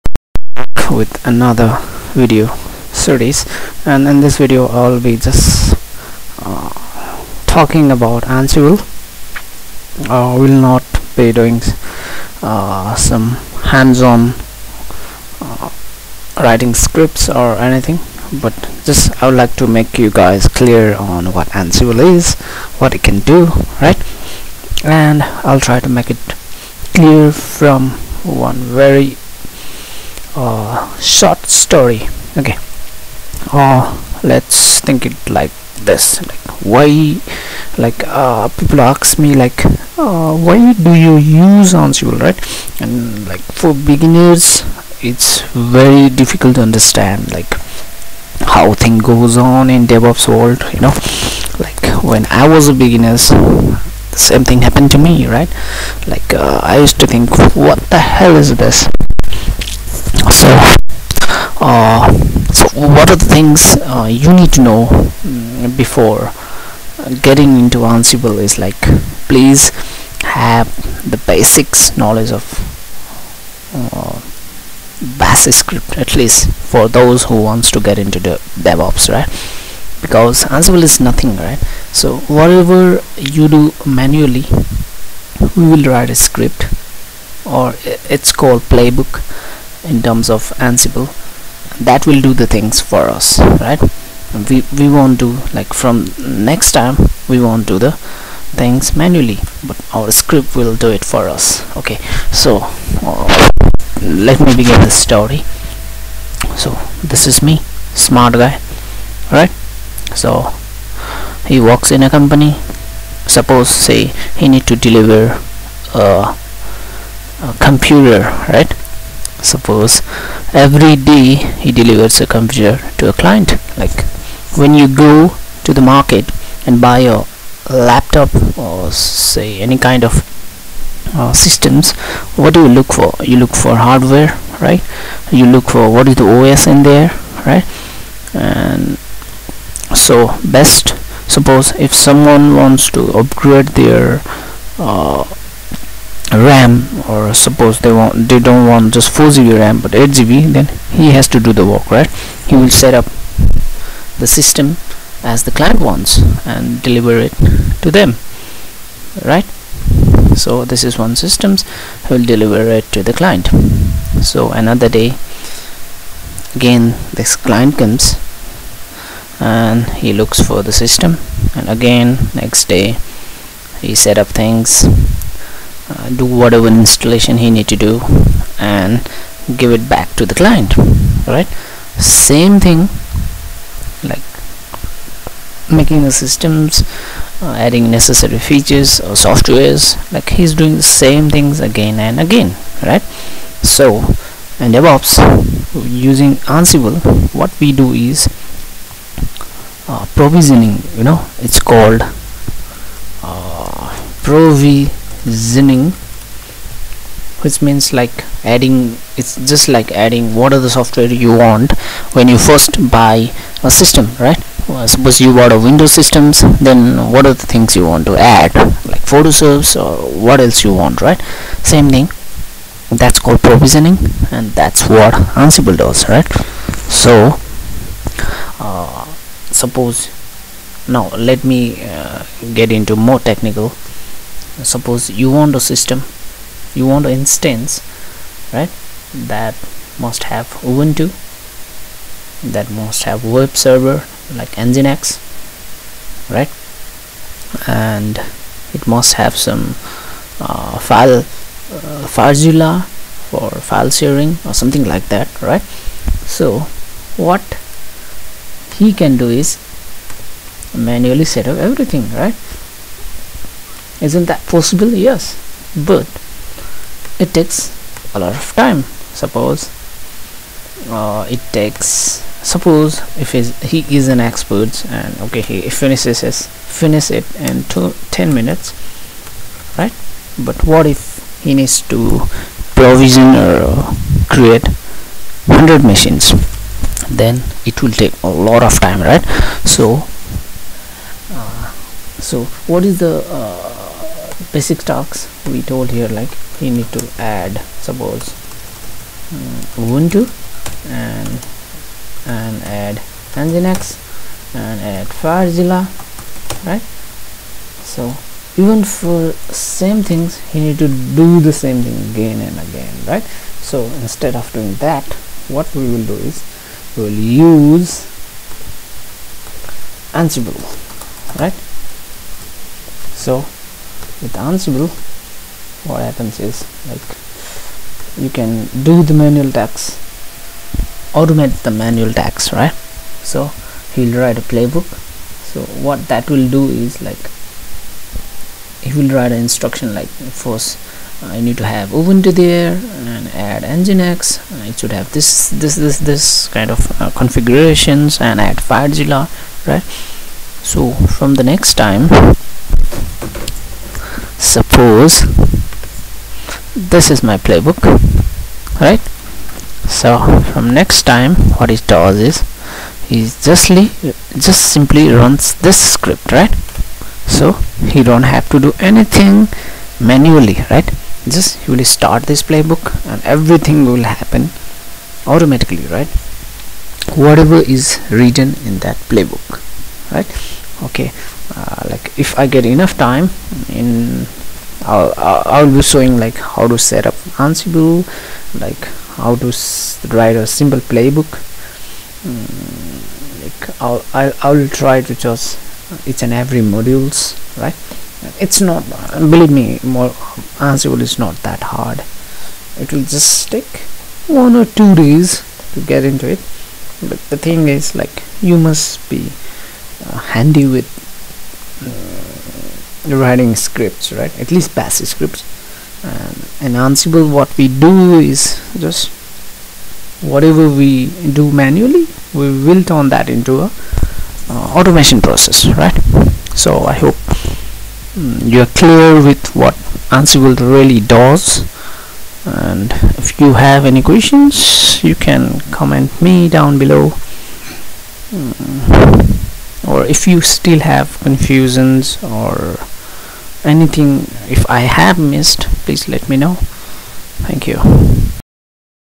understanding ansible in 5 minute with short stories
I know my video quality and accent sucks but believe me I have put all my knowledge and understanding into it. :D
Ansible is not that hard if you have the basic understanding of scripting. If you know how to do things manually,we just use anisible to automate it writing playbook. Here I am just trying to explain what ansible really does with a real simple example